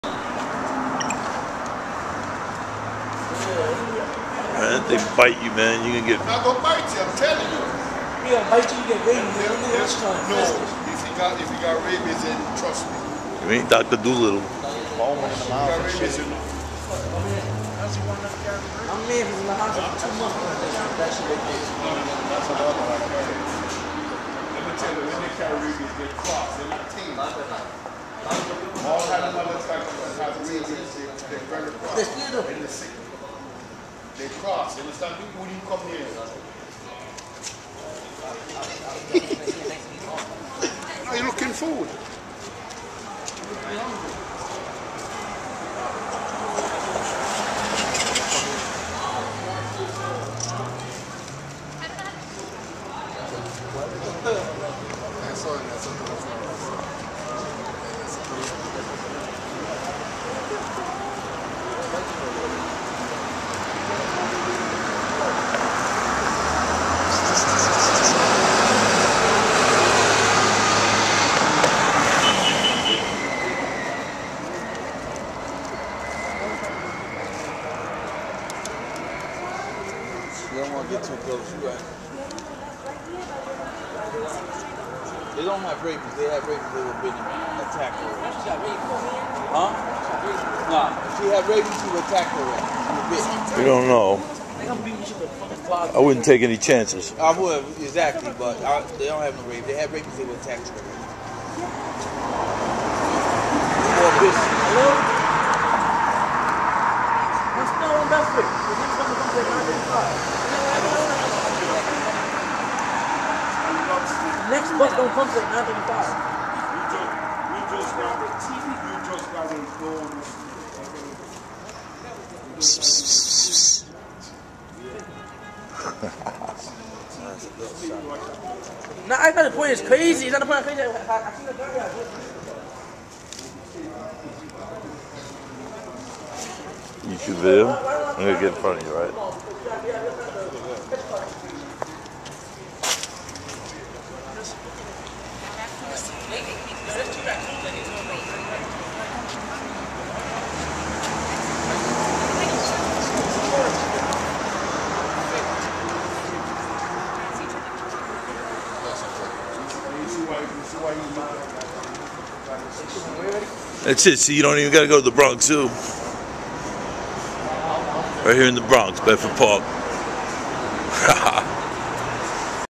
Big Hungry Raccoon Takes Over Bronx Bus Stop
I haven't seen any coons at the 4-train subway station in the Bedford Park section of The Bronx since the fall but now with all this early warm weather they have come out and are foraging for some vittles.  .